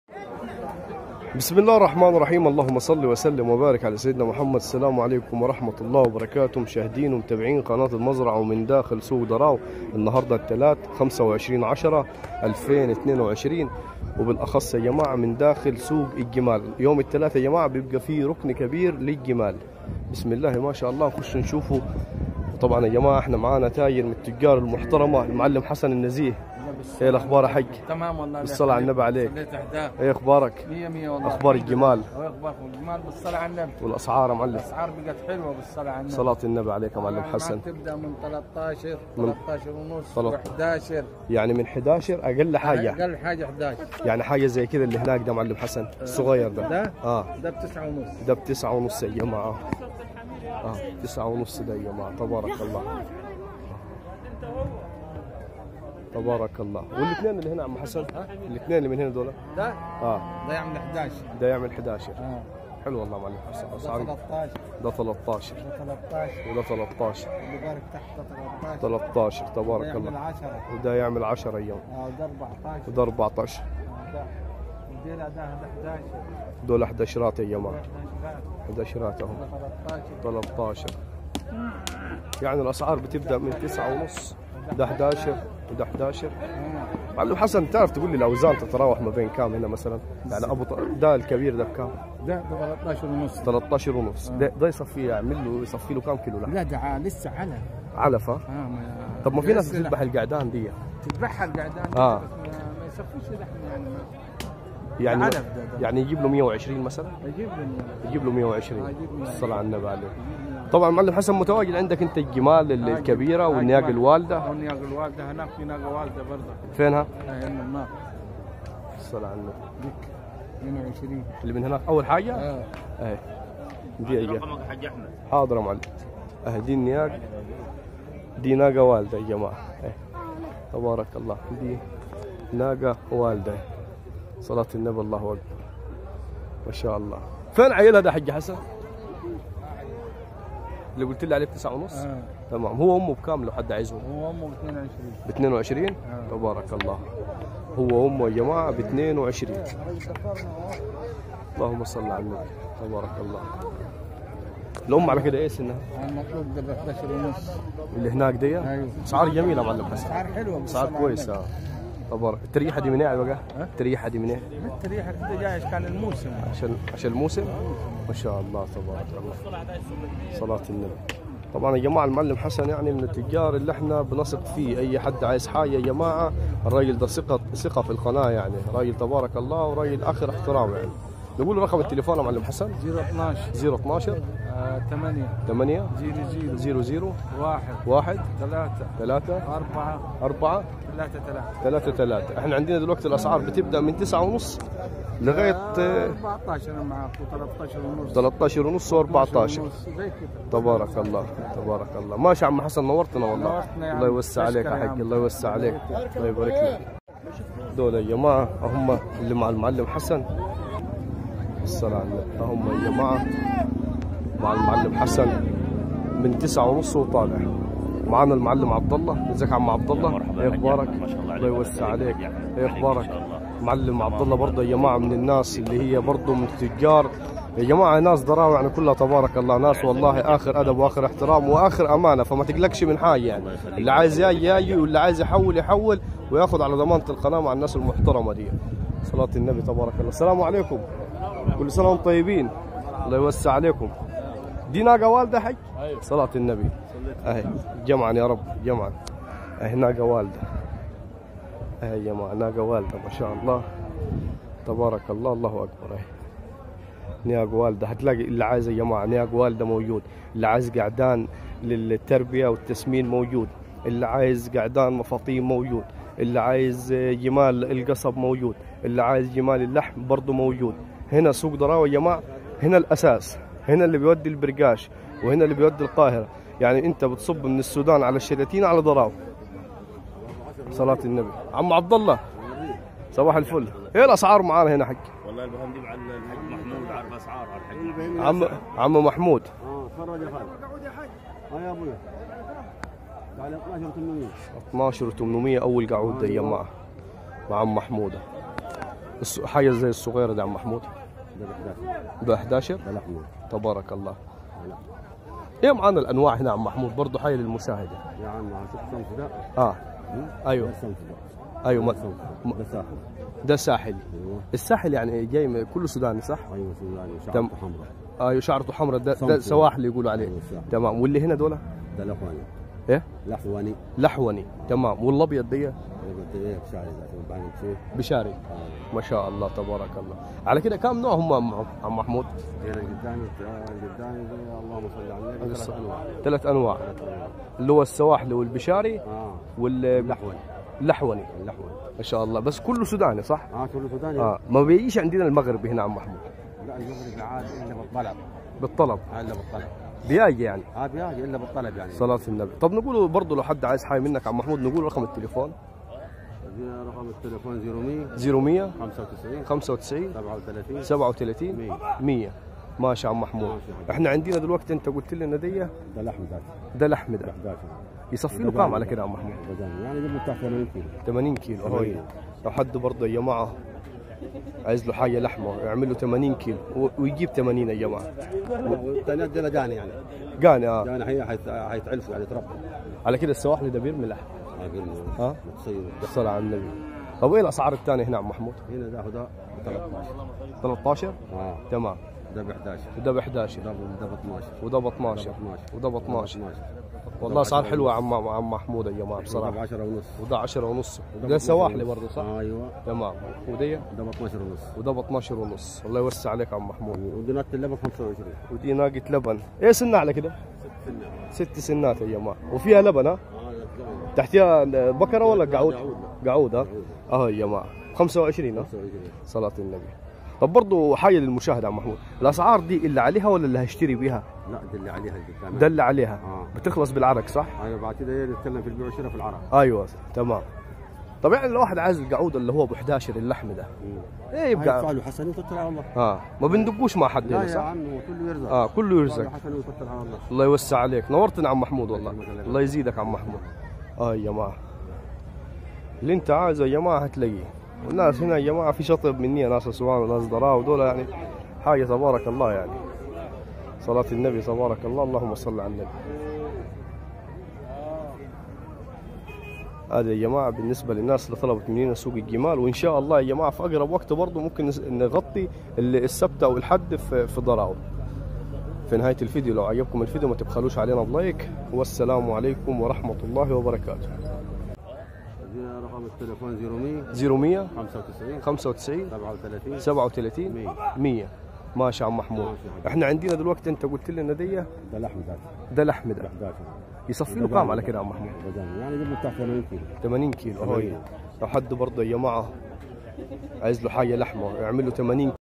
اسعار الجمال والقعدان والنياق الوالده داخل المصدر نفسه بأسعار تنافسية
سوق دراو بمحافظة أسوان